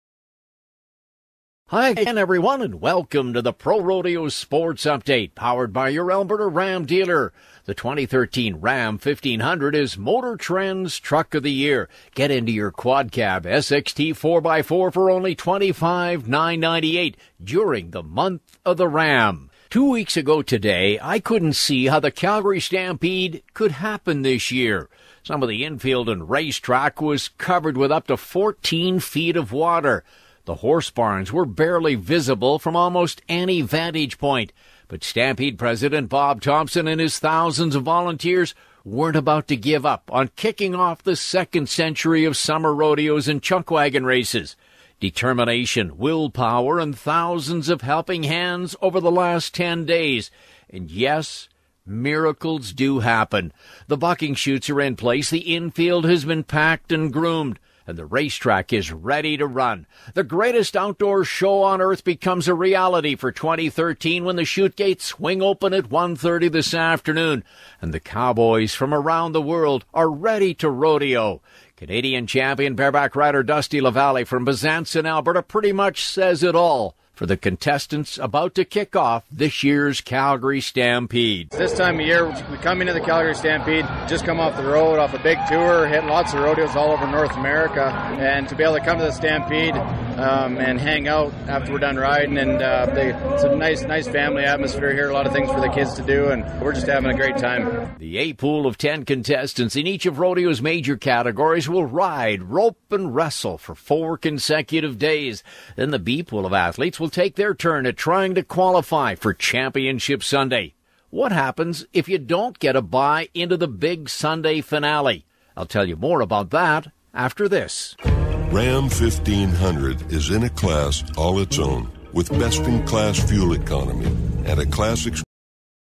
Miracles Happen: Calgary Stampede Opens July 5, 2013 Post Flood
The Calgary Stampede kicks off its rodeo at 1:30 PM July 5, 2013, making what seemed impossible only two weeks ago a reality.

Rodeo report by Arnie Jackson.

Pictures from Debbie Elicksen.